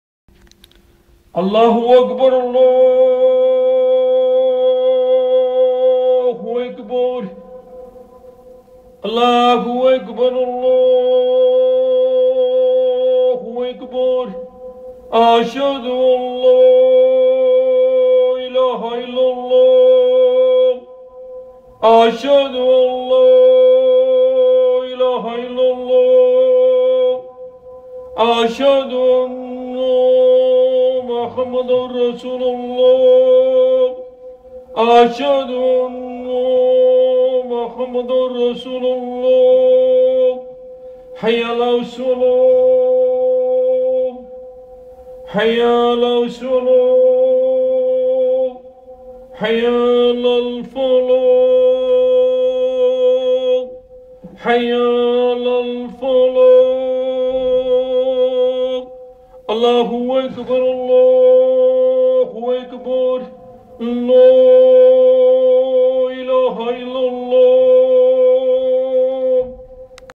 Haji Ghulam Reading Azan at Shah Jahan Mosque 20/12/2012 ( imam Hashmi)
Haji Ghulam Reading Azan at Shah Jahan  Mosque 20/12/2012